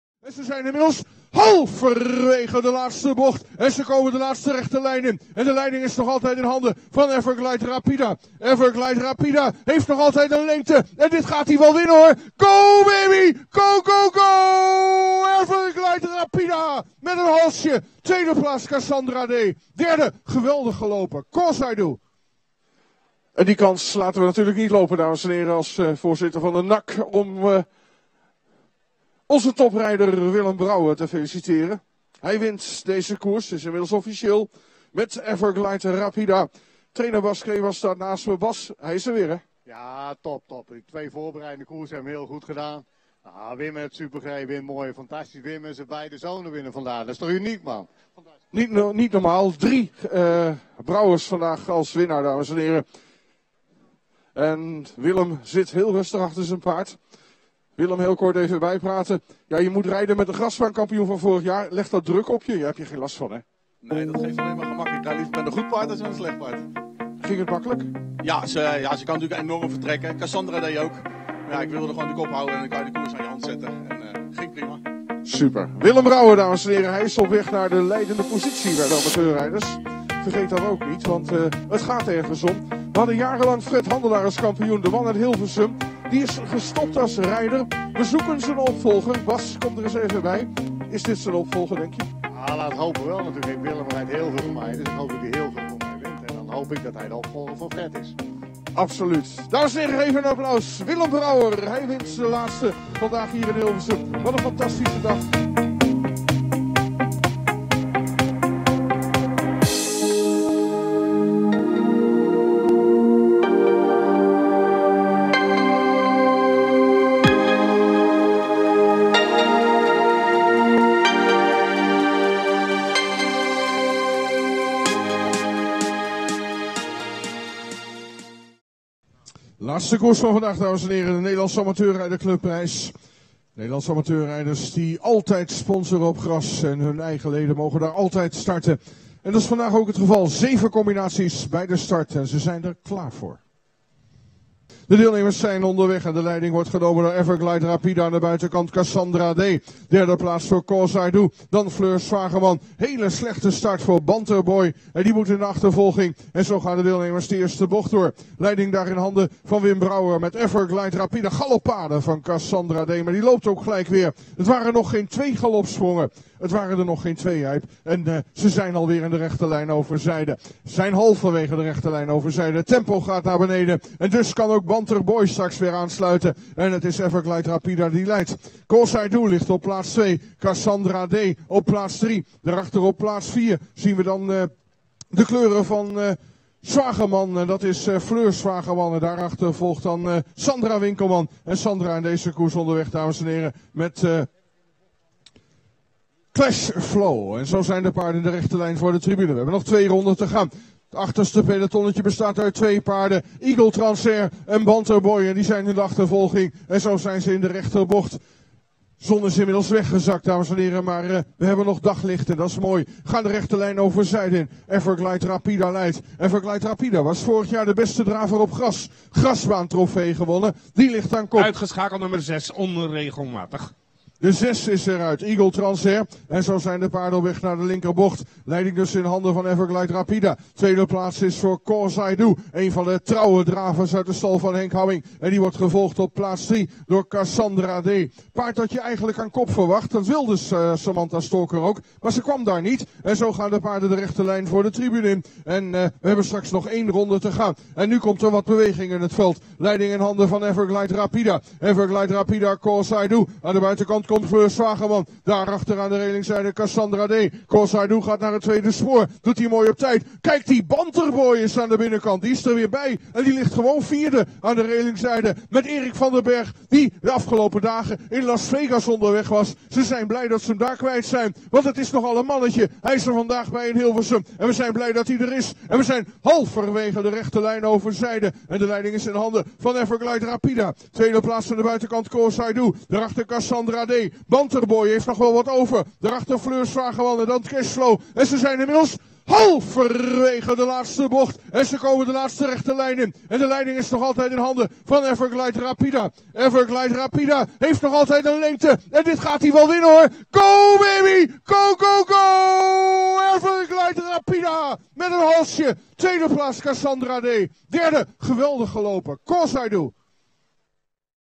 Everglide Rapida & Willem Brouwer winnen op Hilversum - 18 mei 2018
Everglide Rapida & Willem Brouwer winnen voor eig. Gook Crebas en tr. Bas Crebas de Nederlandse Amateurrijders Club-prijs - Hilversum 18 mei 2018